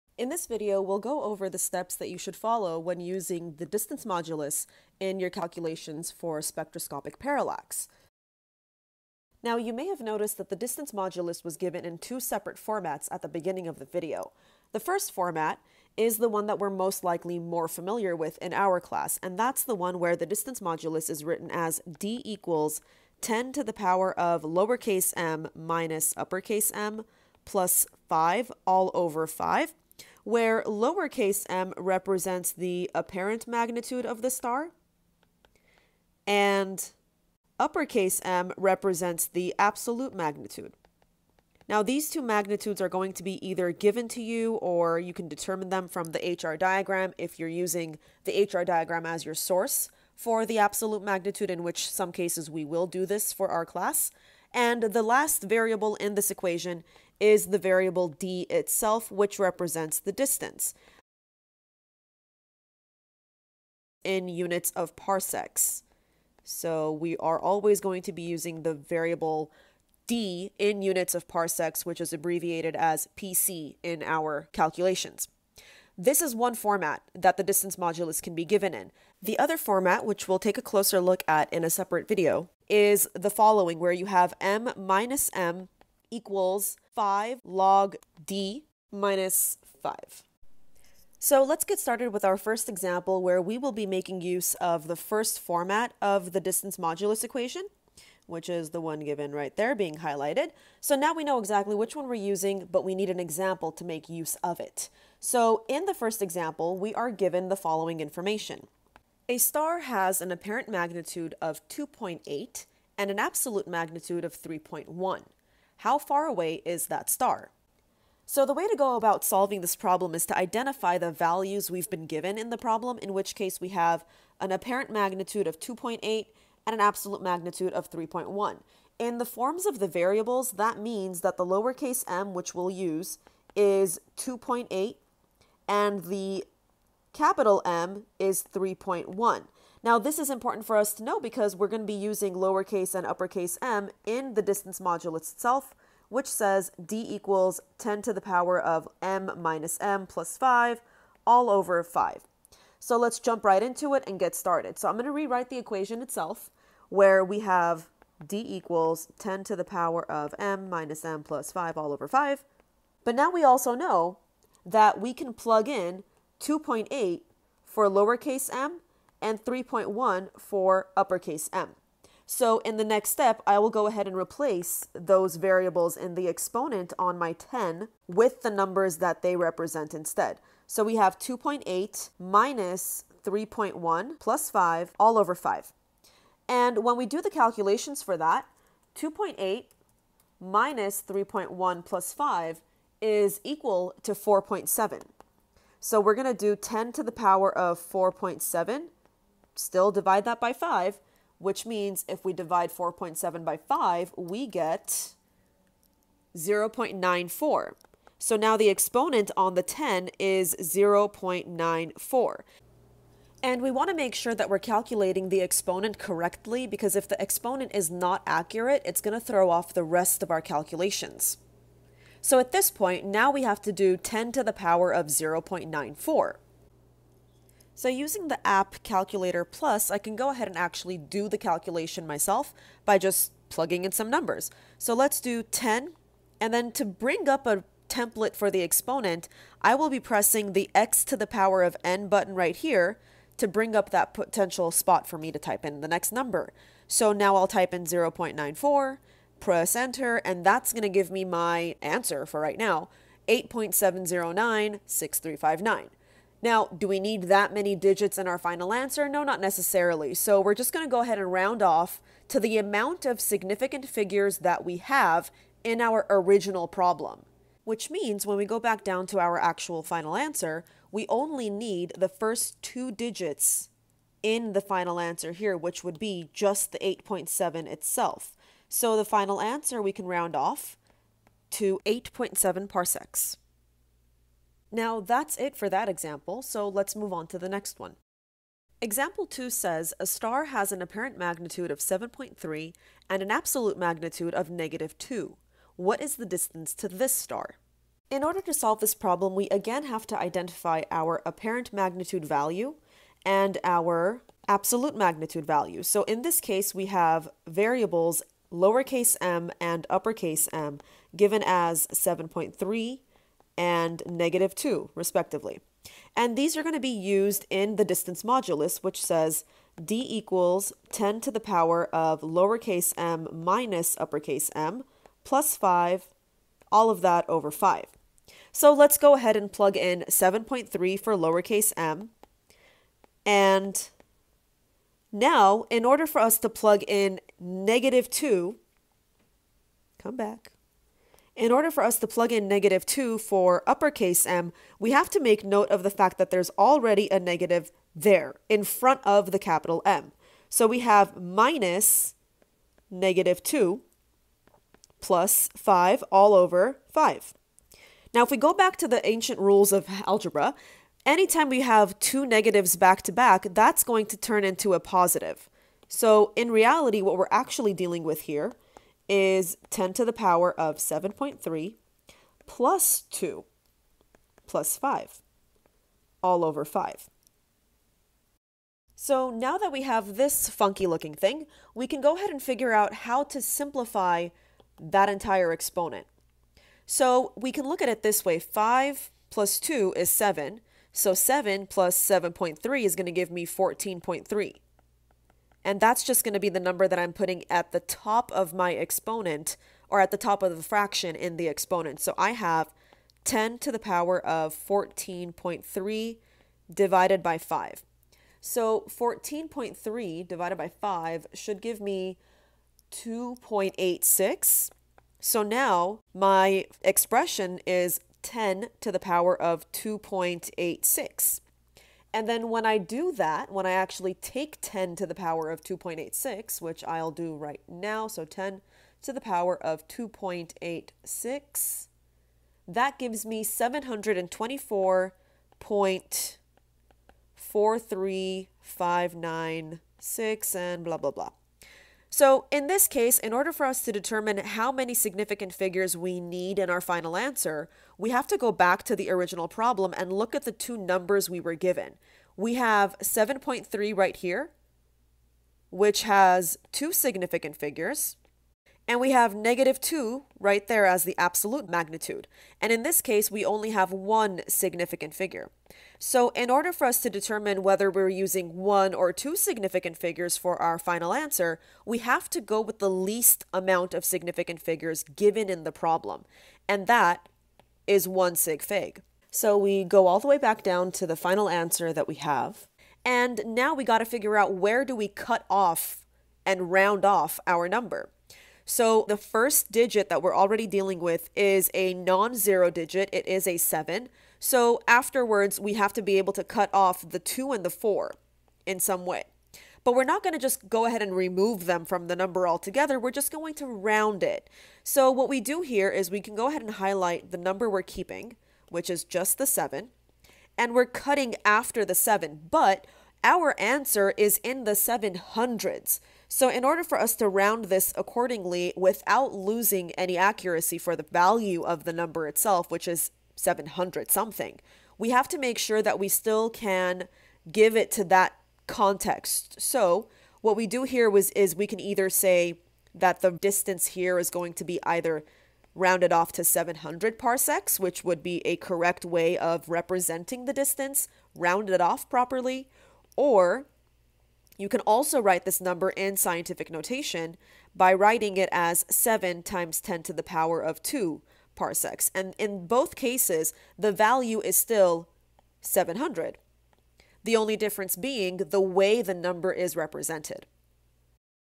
Using the Distance Modulus (2 Examples)
The content in this video is designed and recorded to aid students enrolled in Anoush Kazarians' Astro 120 course at Glendale Community College.

Use this video to help guide you through Part IV of the Unit 7 HR Diagram worksheet (focusing on spectroscopic parallax). It explains how to correctly calculate the distance to stars once you've obtained values for their apparent and absolute magnitudes. In the worksheet, the apparent magnitude is given, but you must estimate the absolute magnitude from your HR Diagram as part of the activity.

Chapters:
0:00 Introduction
1:45 Example 1
4:45 Example 2